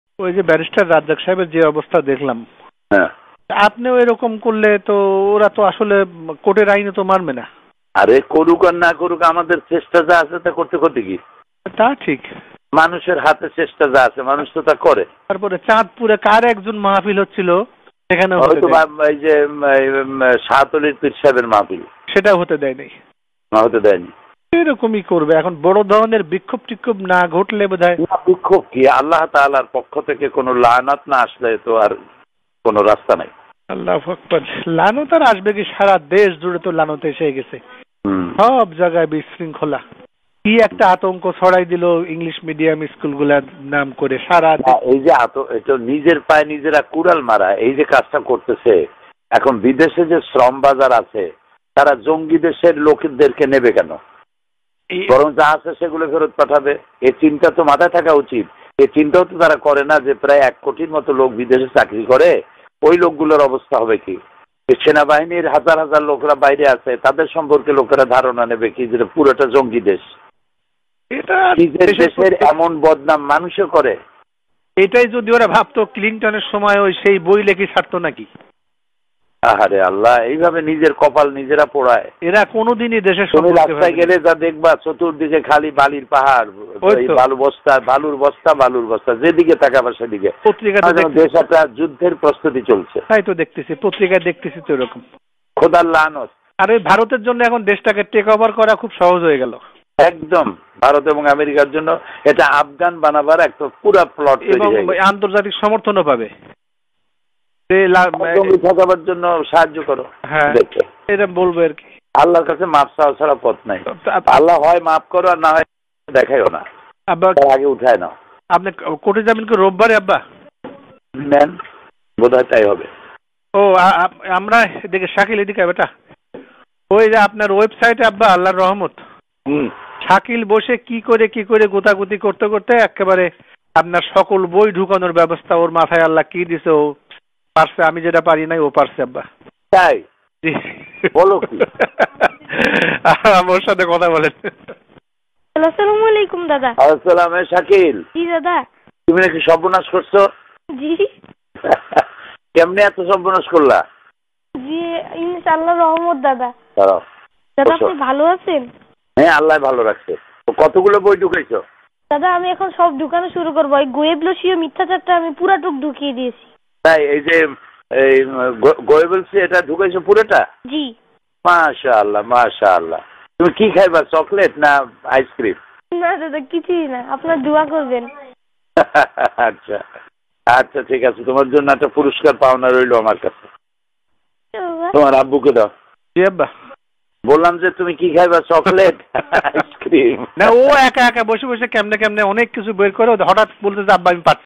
শেখ হাসিনার উপর আল্লাহর লানত কামনা করেছিলেন দেলাওয়ার হোসাইন সাঈদী । Top News BD । Sayeedi
শেখ হাসিনার উপর আল্লাহর লানত কামনা করেছিলেন দেলাওয়ার হোসাইন সাঈদী, জেলে থাকা অবস্থায় পরিবারের সাথে তার কথা বলার একটি অডিও সম্প্রতি ফাঁস হয়েছে